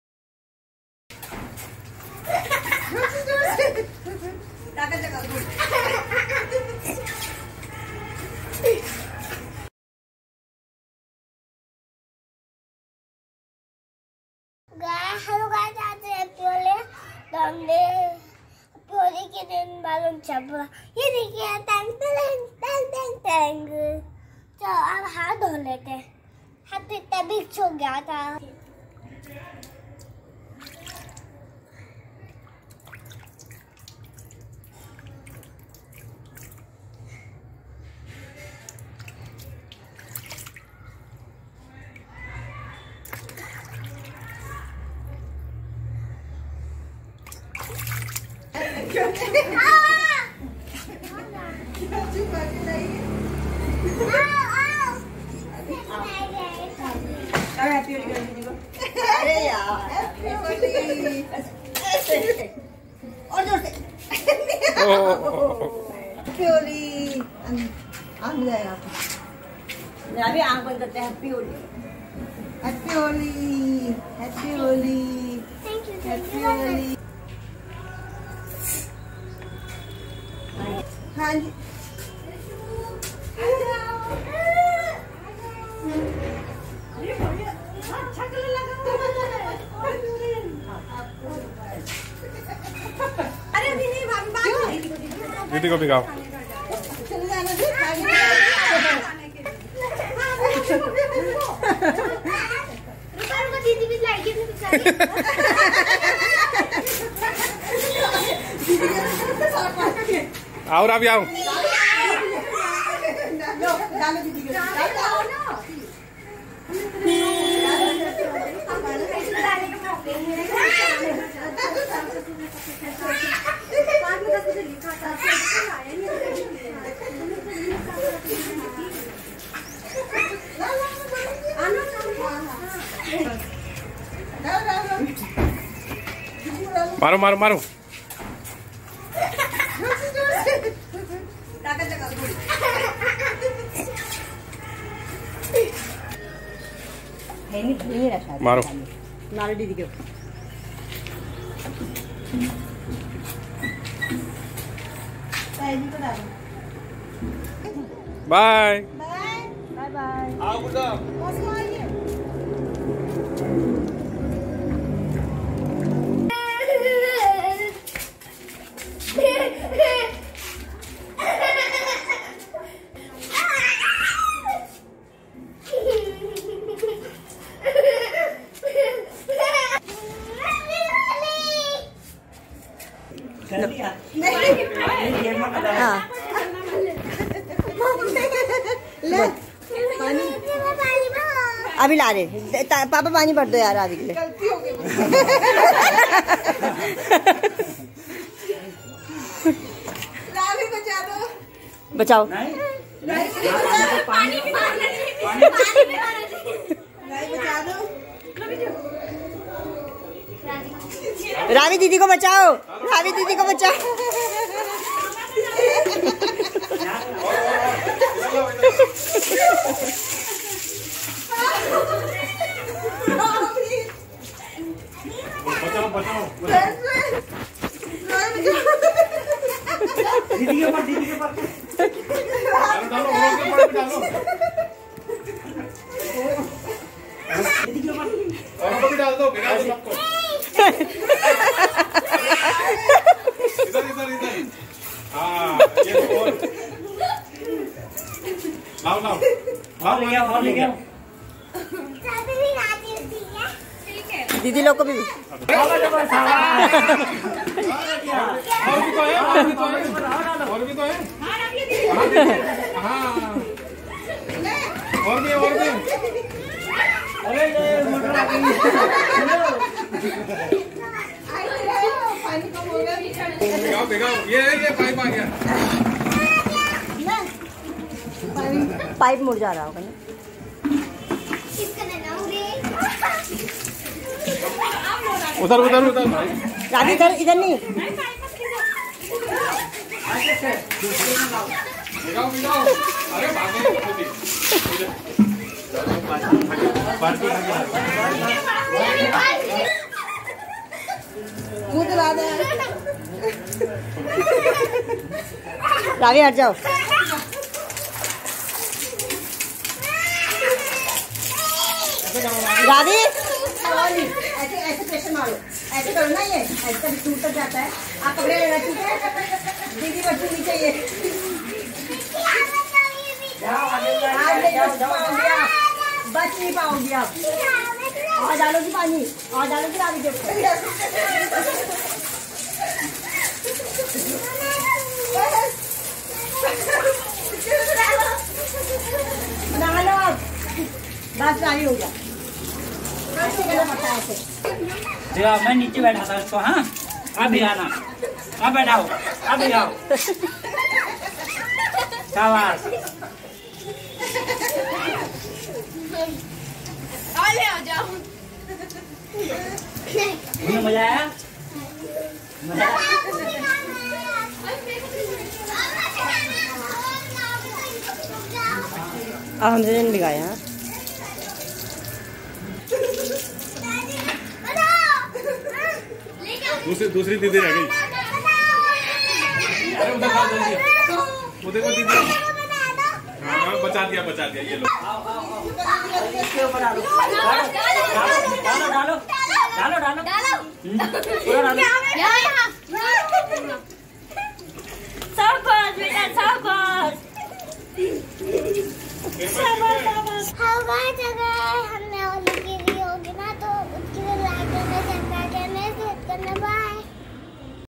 Happy Holi 2022 | रावी की होली का पहला vlog | Kumaoni Holi | Haldwani
Hi,

I am a three year old girl from India. I enjoy acting and love animals. I also love playing,